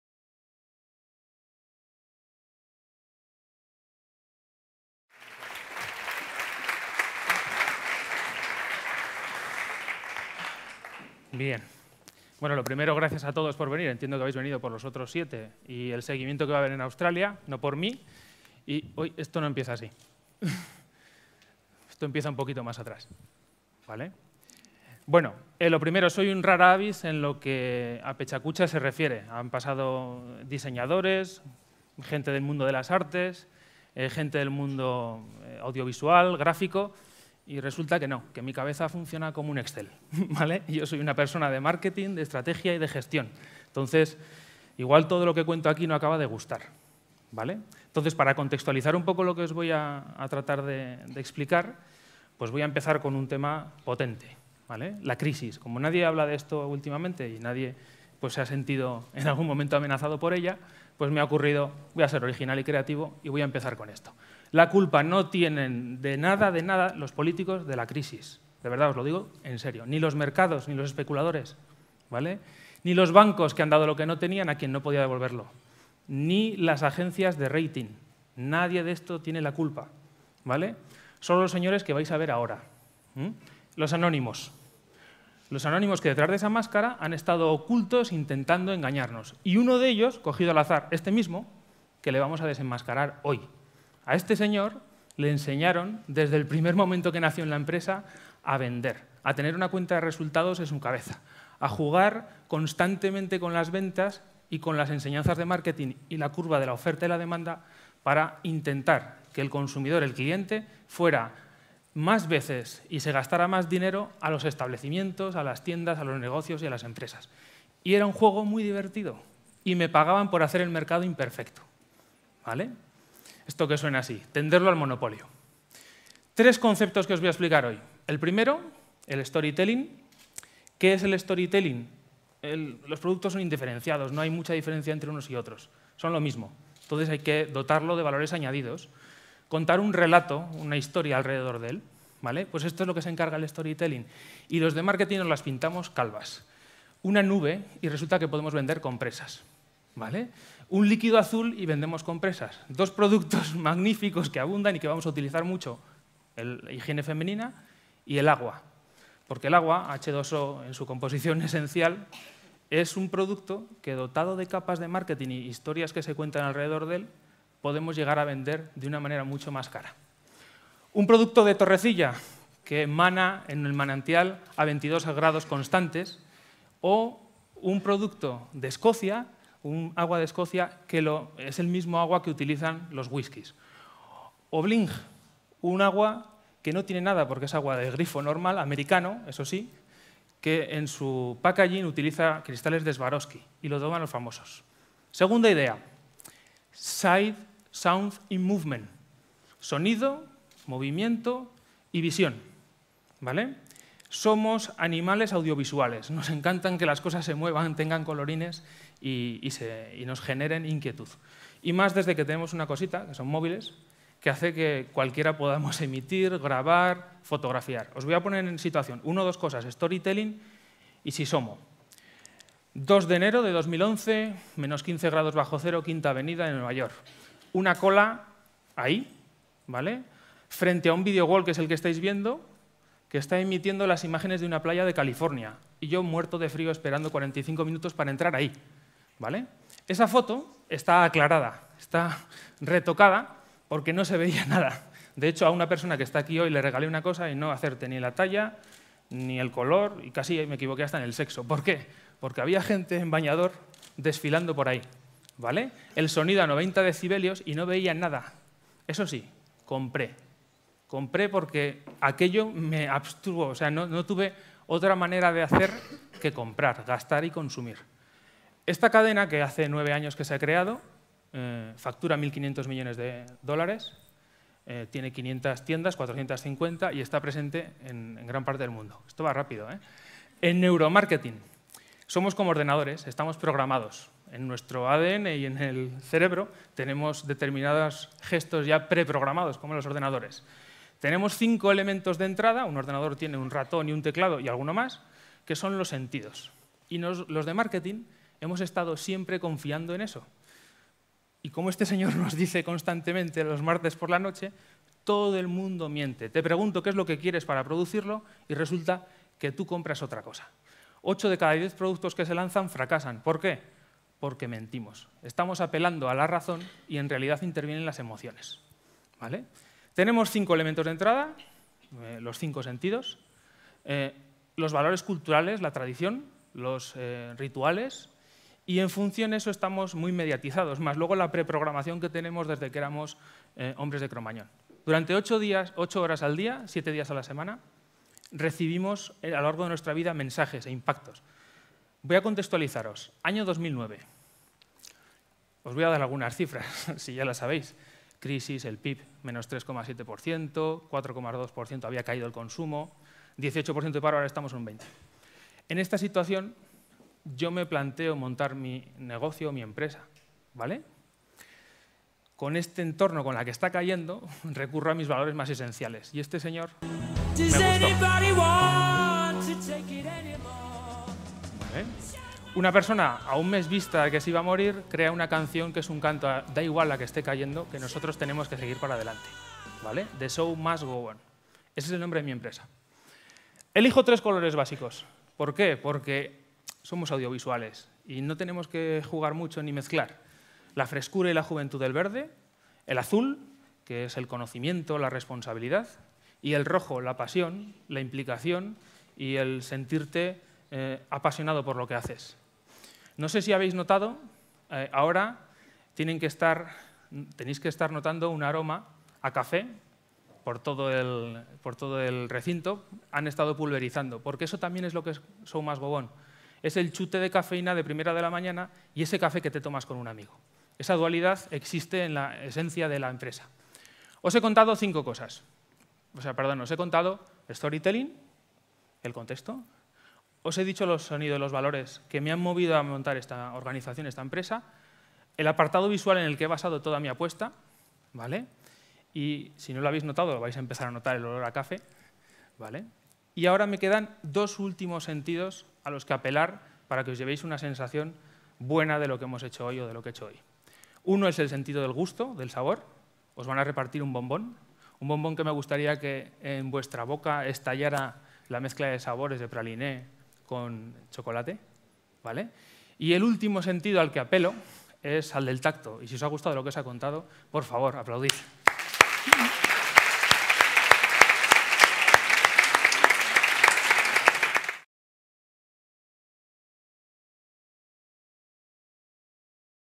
Pecha Kucha TSMGO
Vídeo de la ponencia de Ricardo Moreno, CEO de The Show Must Go On, en Pecha Kucha Night Logroño que se celebró el día 29 de septiembre de 2011. Este encuentro, que tuvo lugar en el Sala Gonzalo de Berceo, contó con la participación de ocho emprendedores que compartieron sus proyectos, sus ideas y sus trabajos con los asistentes en el formato 20x20 de Pecha Kucha.